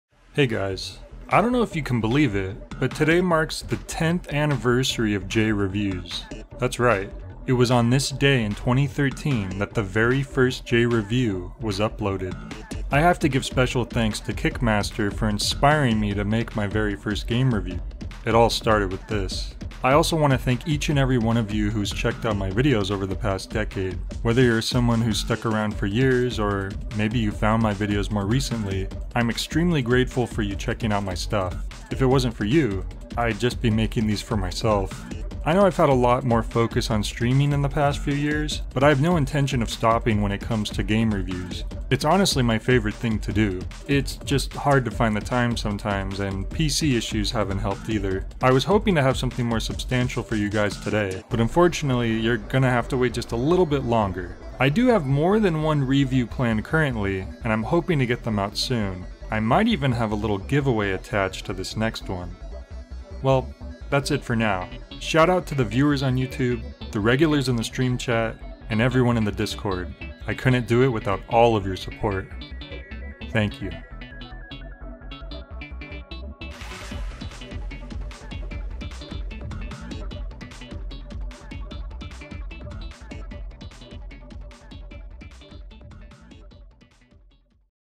Jay Reviews 10th Anniversary Thanks!!
A sincerest thanks to all of you for keeping me going for 10 years!
More reviews to come!